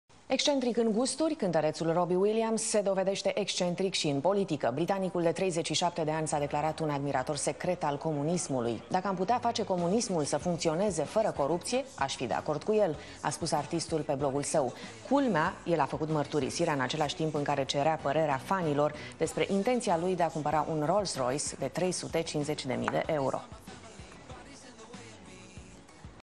Robbie Williams s-a declarat un admirator secret al comunismului 29 IULIE 2011
Excentric in gusturi cantaretul Robbie Williams se dovedeste excentric si in politica. Britanicul de 37 de ani s-a declarat un admirator secret al comunismului. -ampquotDaca am putea face comunismul sa functioneze fara coruptie as fi de acord cu...